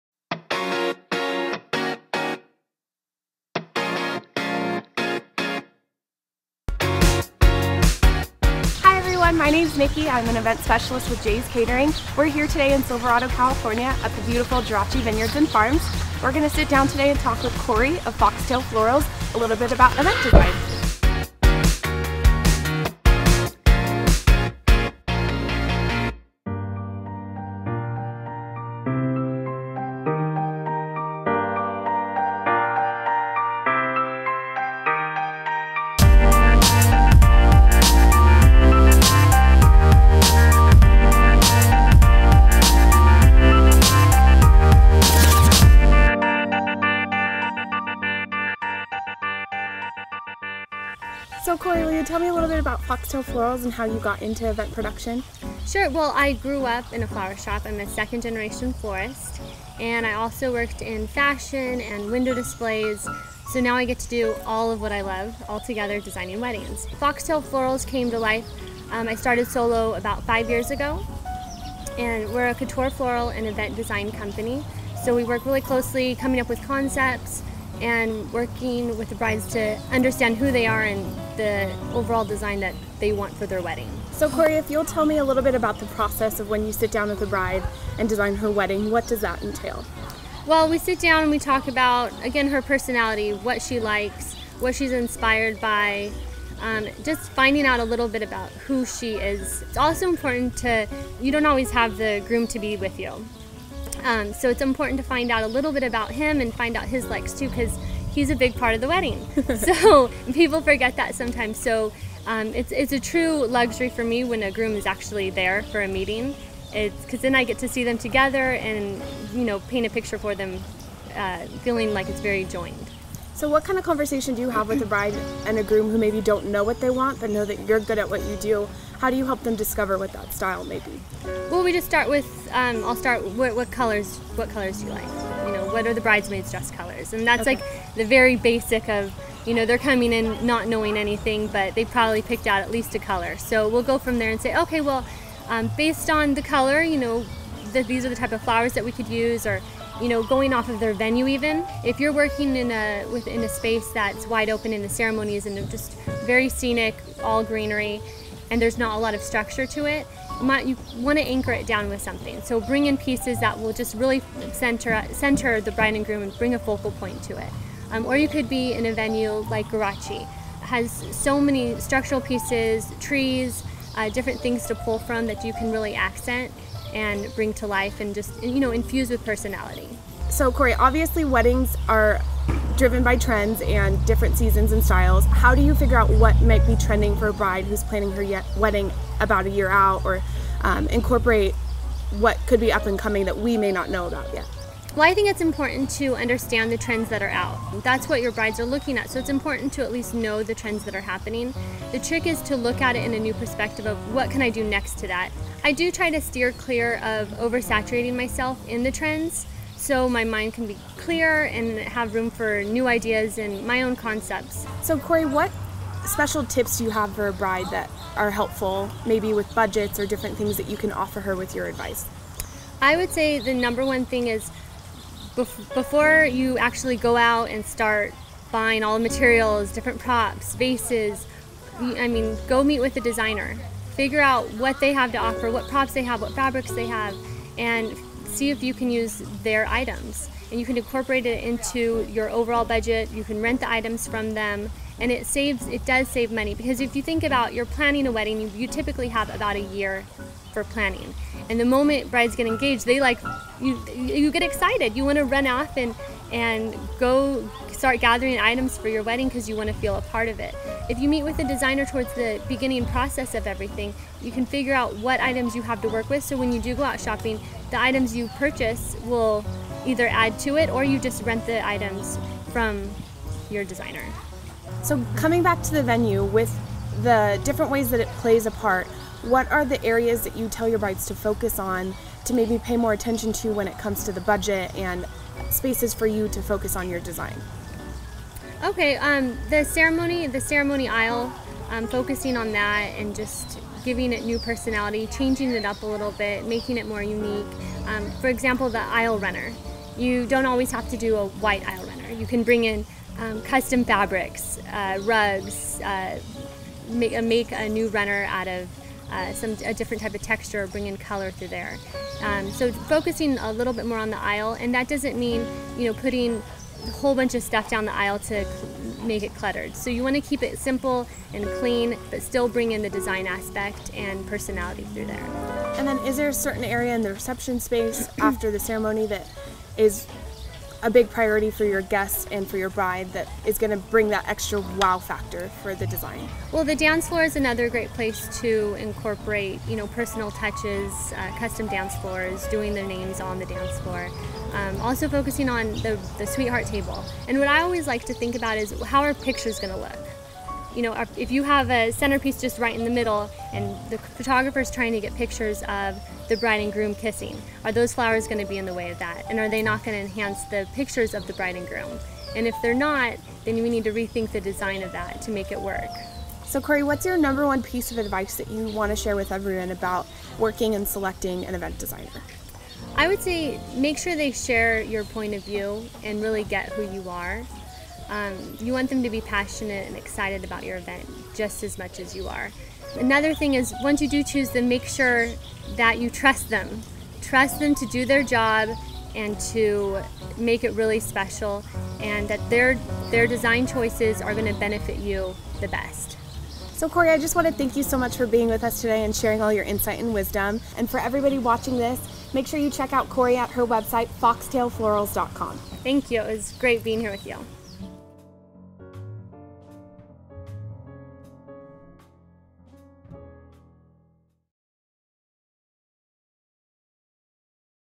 Eventful: Designing with Foxtail Florals
Ready to make your week a little bit more "Eventful"? Good—so are we! Our latest episode is all about designing special events, filmed at the spectacular Giracci Vineyards and Farms. Listen to Foxtail Florals' design guru, Corie, give some helpful tips + insight into the world of event design